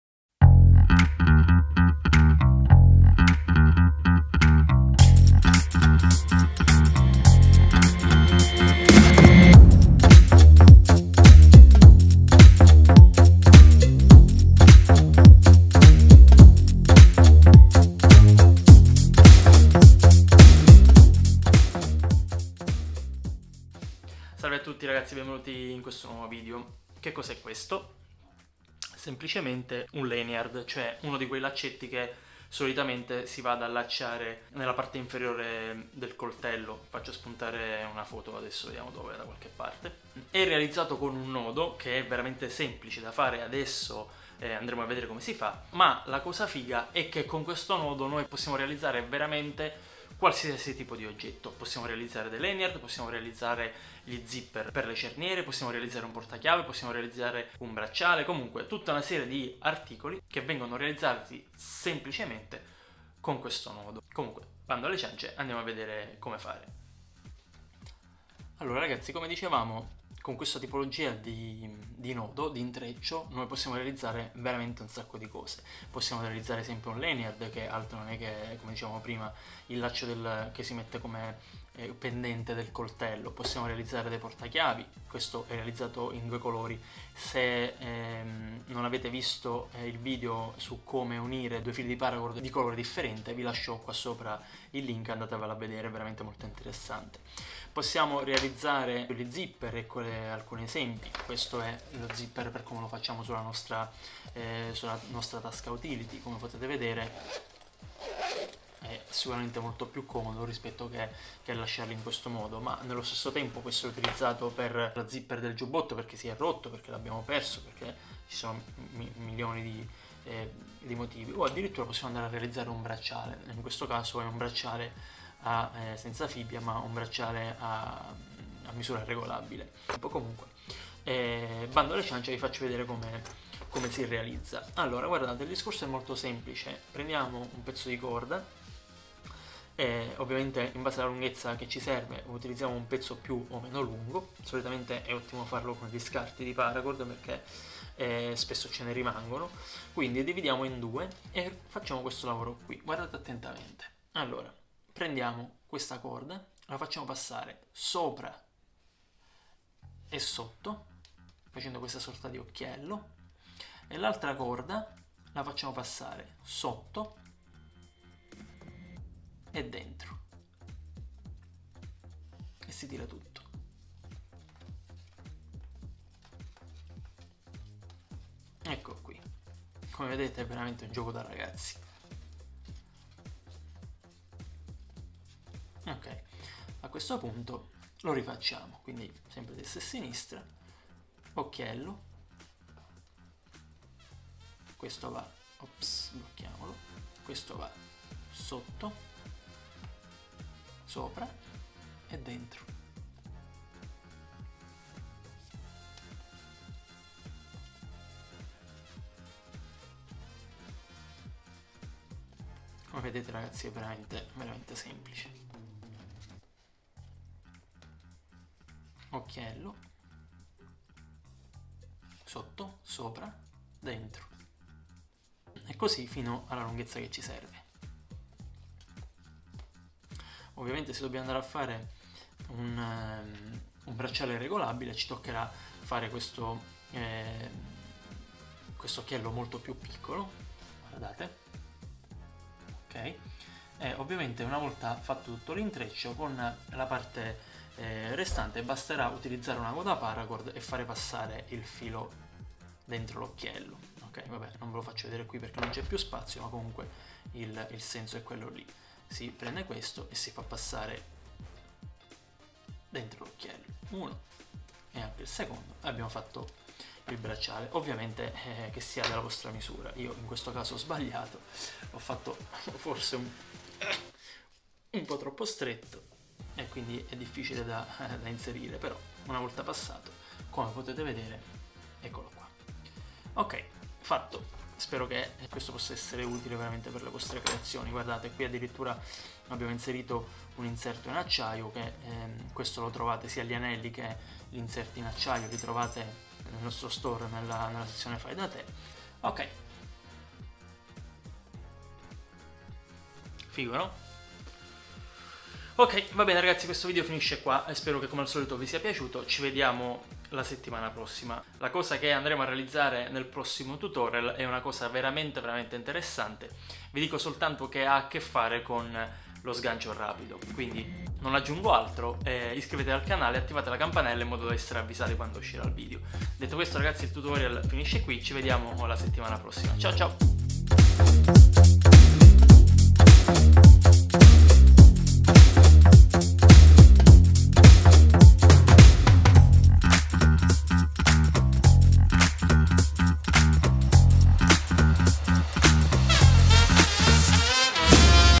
TUTORIAL PARACORD: un nodo per MILLE utilizzi!! tutorial #10 Blacksquirrel
Un intreccio per tantissimi utilizzi, zipper, portachiavi, lanyard per coltelli, bracciale ecc | Tutorial Italiano

CAPITOLI:
0:00 Intro
0:22 Presentazione
1:06 Prima del tutorial
2:09 Tutorial
4:59 Conclusioni
In questo video sono presenti inserimenti a scopo commerciale.